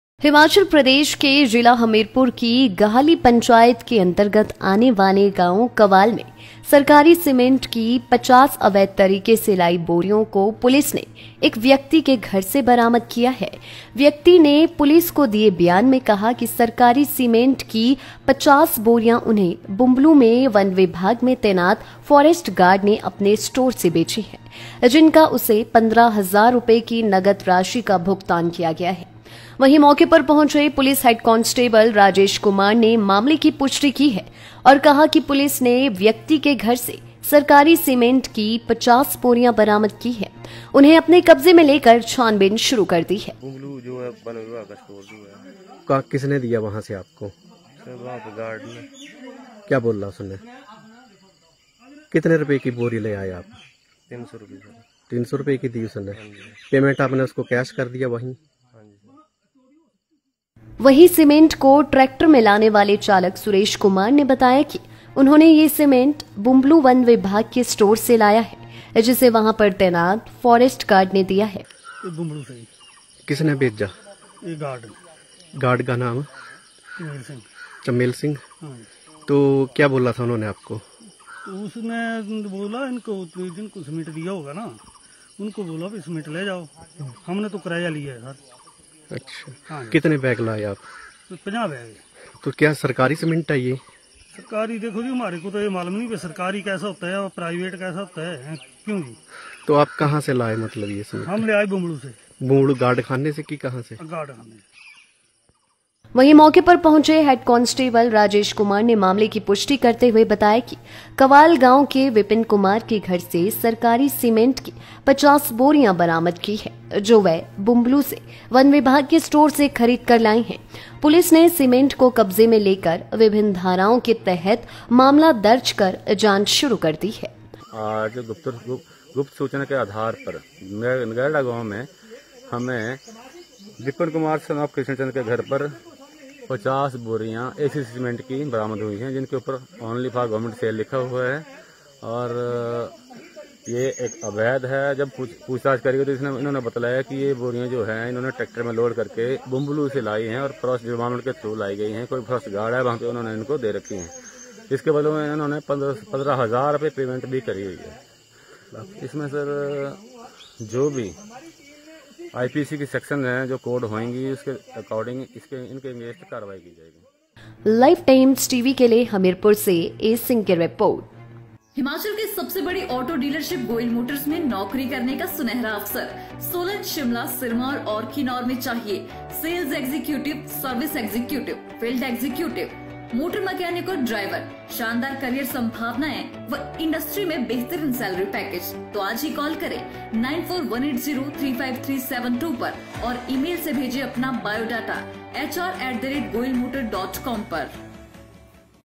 हमीरपुर : घर से बरामद की सरकारी सीमेंट की बोरियां।
Hamirpur: Government cement sacks recovered from the house.